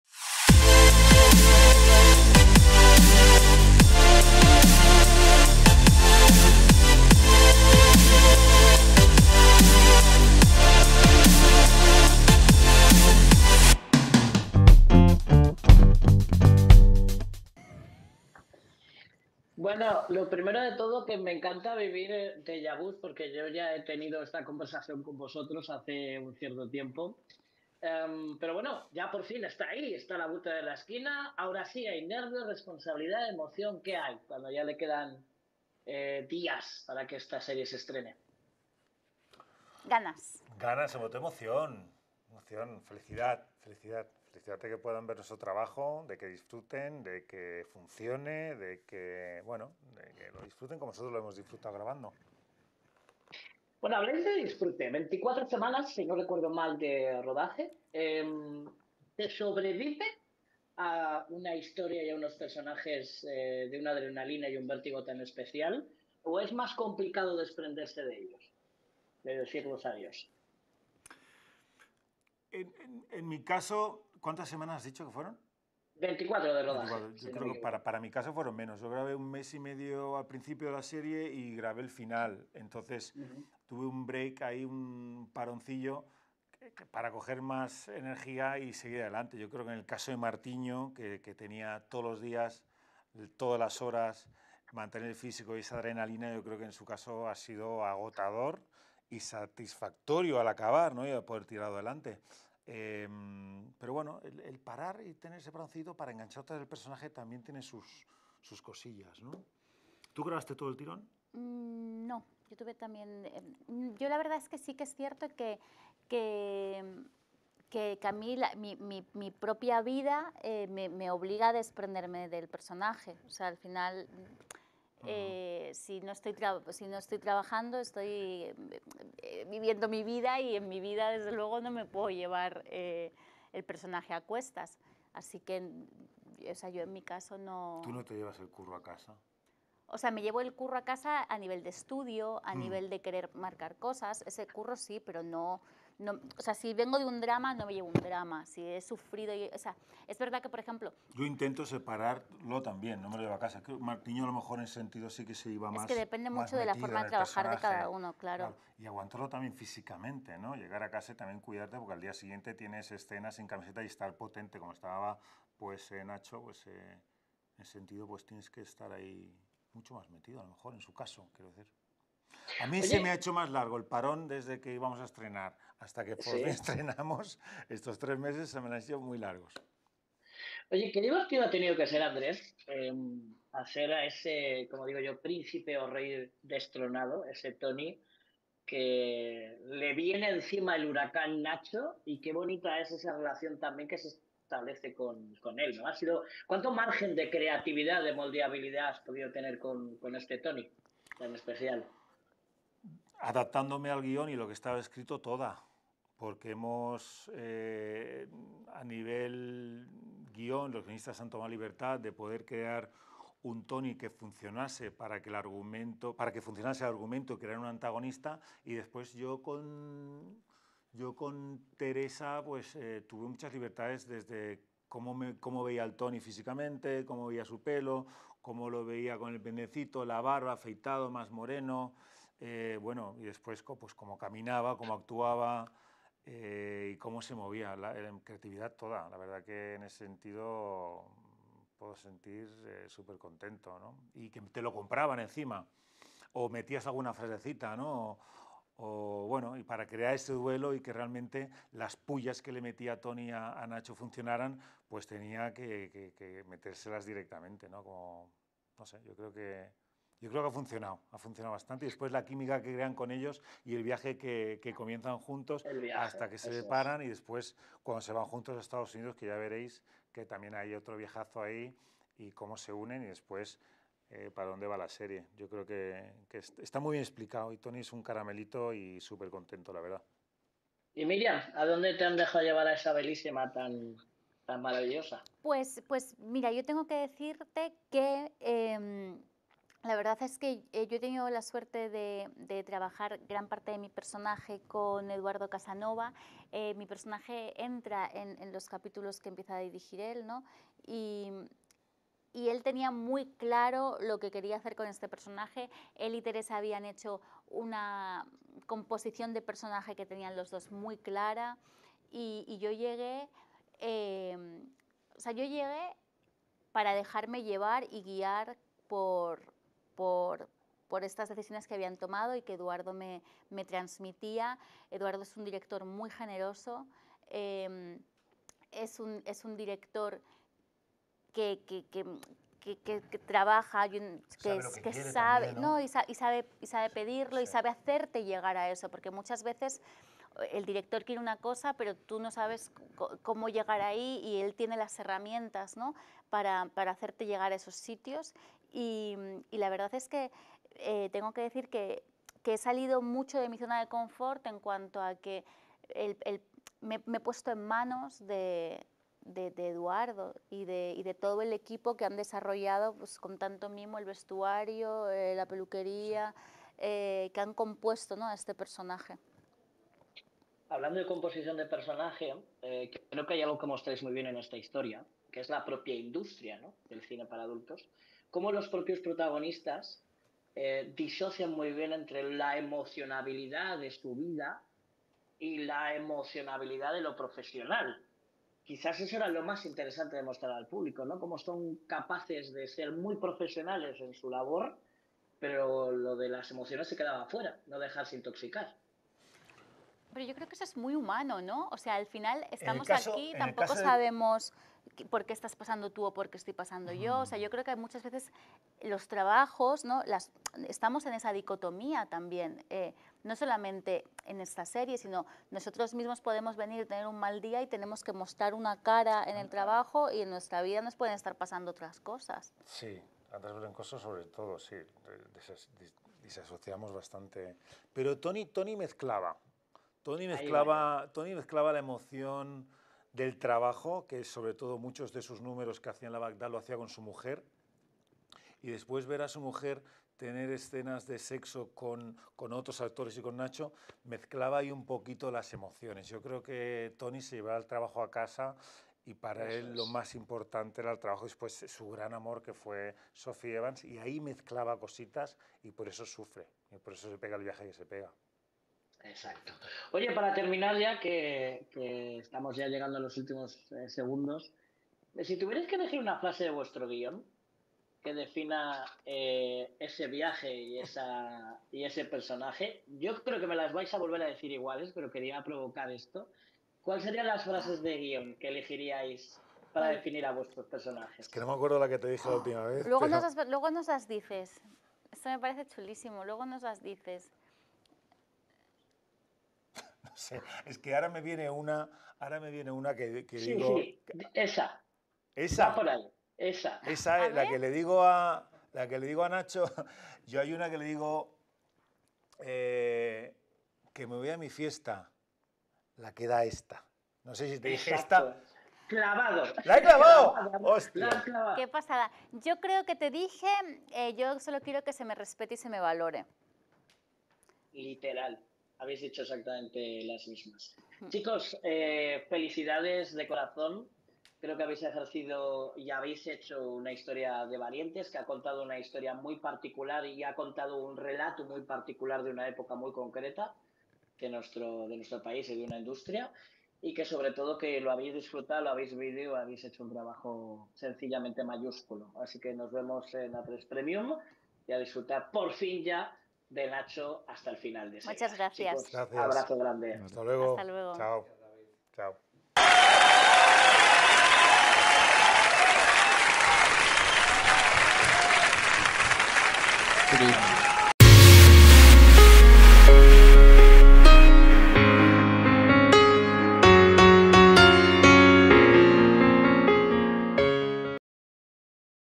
ANDRÉS VELENCOSO y MIRIAM GIOVANELLI nos presentan "NACHO" : "Tuvimos LIBERTAD con LOS PERSONAJES"
ANDRÉS VELENCOSO y MIRIAM GIOVANELLI nos presentan "NACHO" : "Tuvimos LIBERTAD con LOS PERSONAJES". Descubre lo que nos contaron.